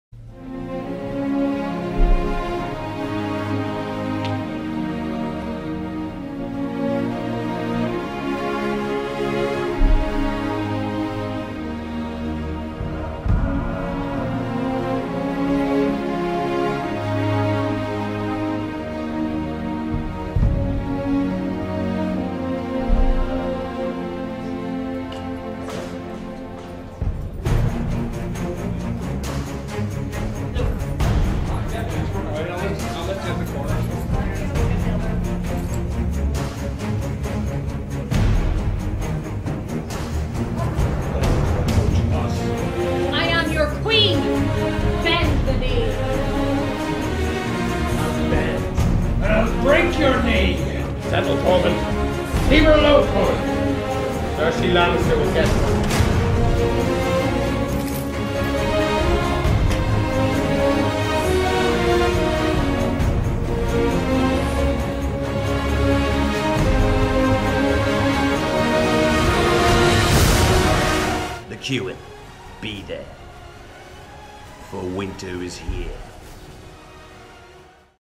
Game Of Thrones Banquet at The Cuan
Come and enjoy our Game Of Thrones Banquet here at The Cuan in Strangford beside Winterfell and many more film location sites, it is a great way to spend an evening.